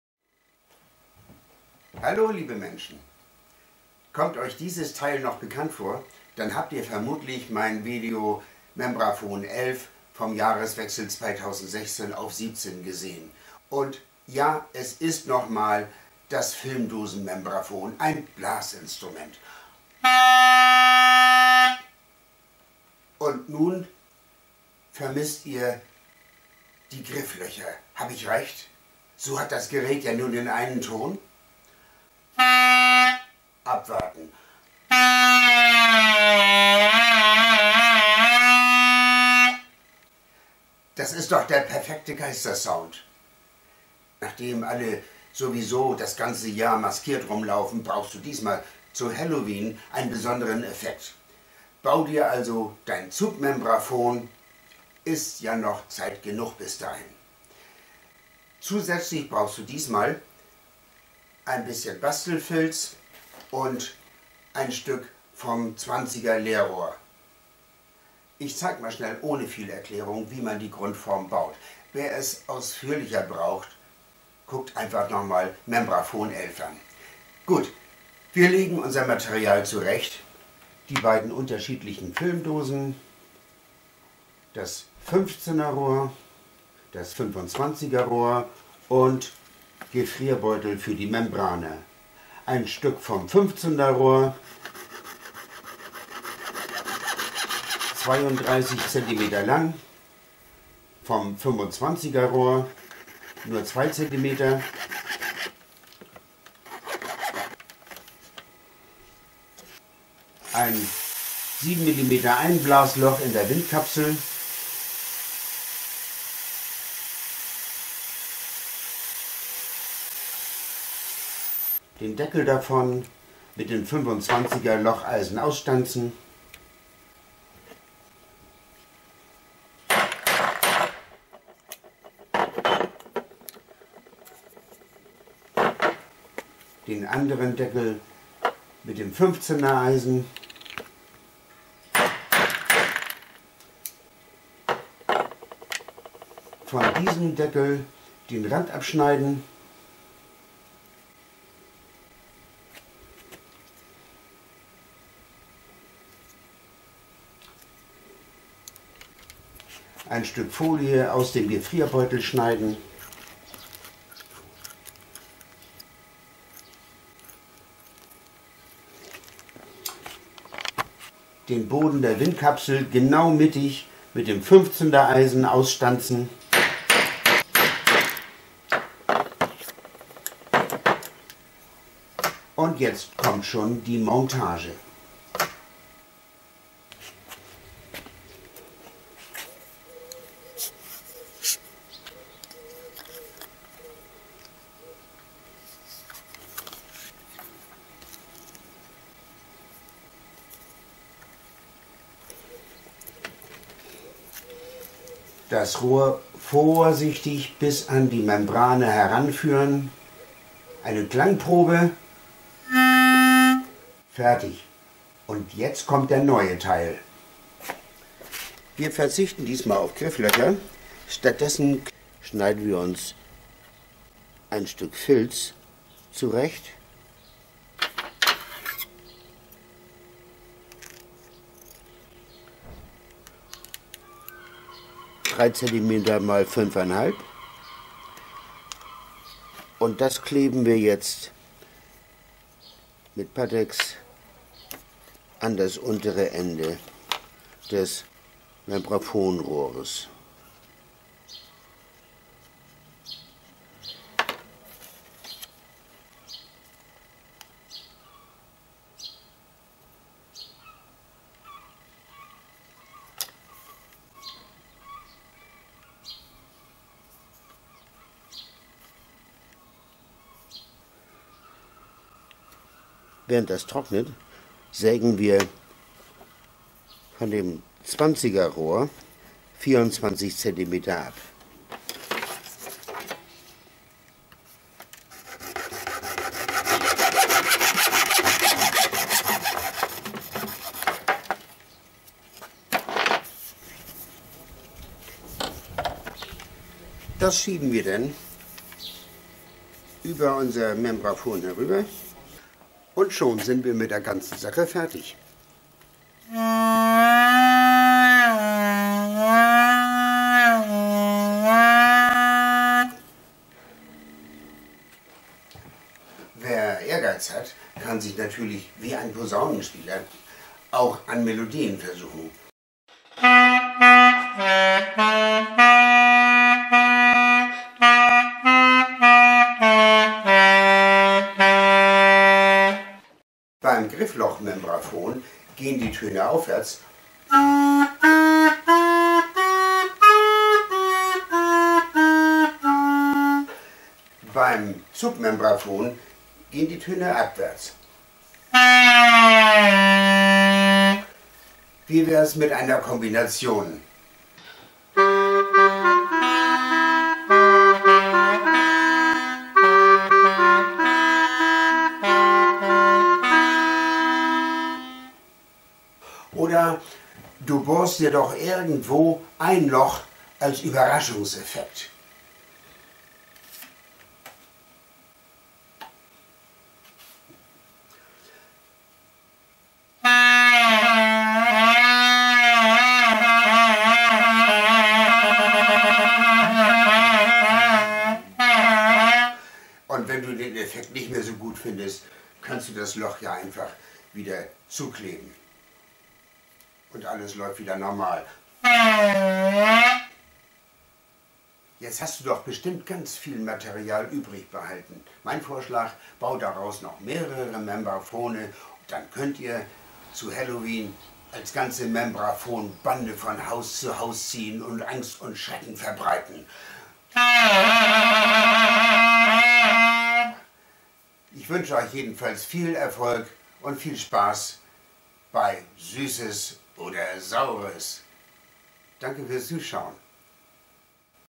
Membrafon 12: Das Zugmembrafon
Meine Maßangaben sind nur Vorschläge. Probiere auch gerne andere Rohrlängen aus!
In Mebrafon 11 hab ich ein 14er Locheisen verwendet. Diesmal kommt stattdessen ein 15er Locheisen zum Einsatz, das passt dann doch besser zum 15er Leerrohr.
Von diesem Zugmembrafon wurden in einer Ferienspaß-Aktion mehrere Dutzend gebaut. Die Kids waren zwischen 8 und 12 Jahre alt.